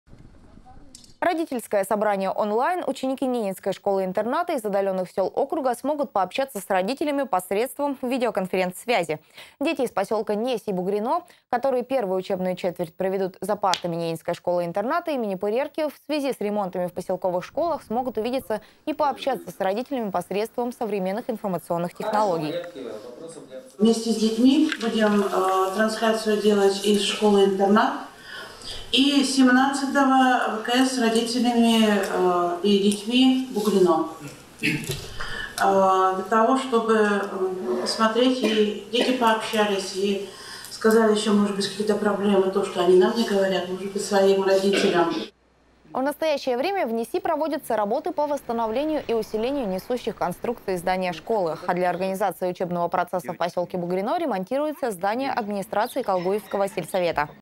Ученики НШИ смогут общаться с родителями по видеоконференцсвязи
Дети из села Несь и поселка Бугрино, которые в связи с ремонтами в поселковых школах первую учебную четверть проведут за партами Ненецкой школы-интерната имени А.П. Пырерки, смогут увидеться и пообщаться с родителями посредством современных информационных технологий.